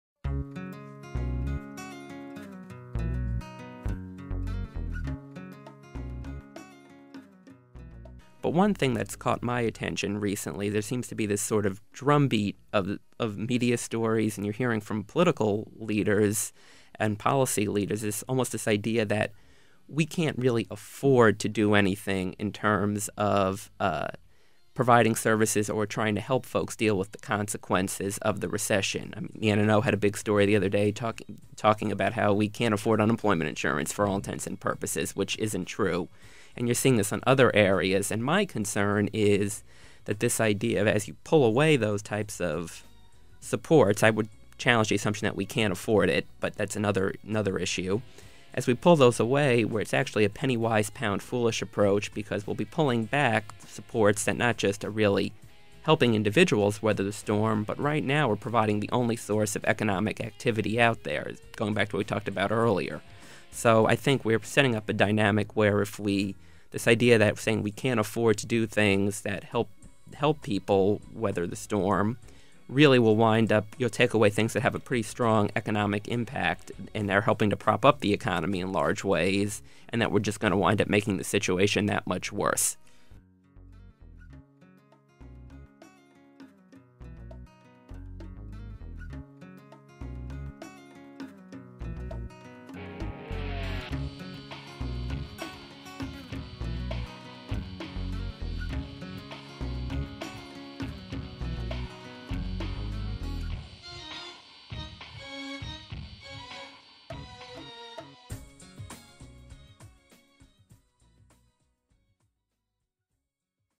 John Quinterno on NC's high unemployment rate.wmv
John Quinterno of  South by North Strategies discusses the problems of scaling back support services and accepting North Carolina's high unemployment rate.